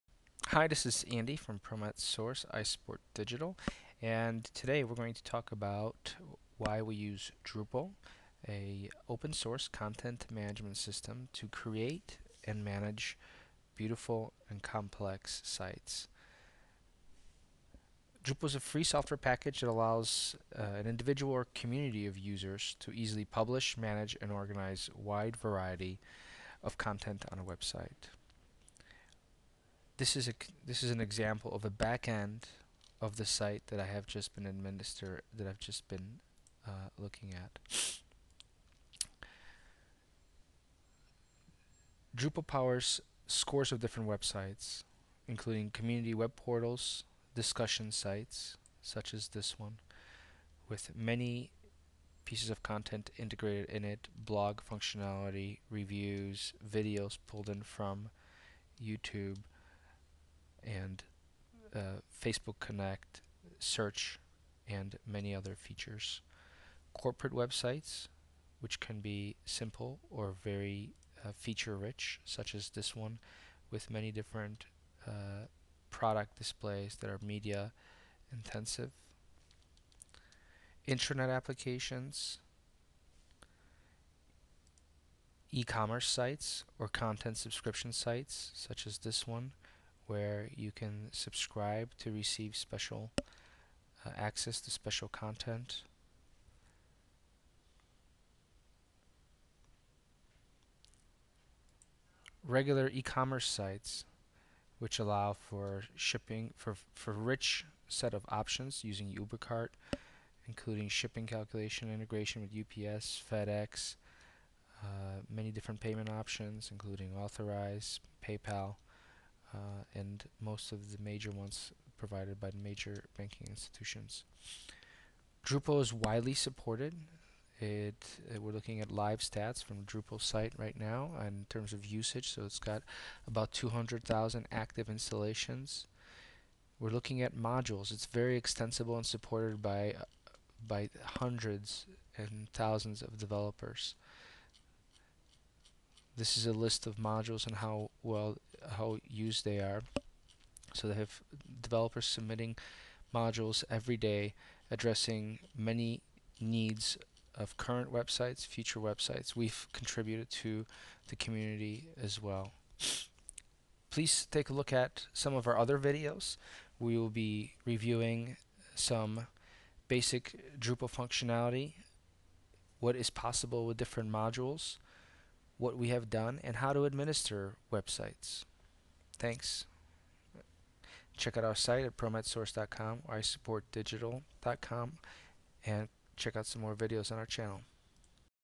Why use the Drupal CMS?
Drupal CMS overview, how its used and some examples of our drupal implementations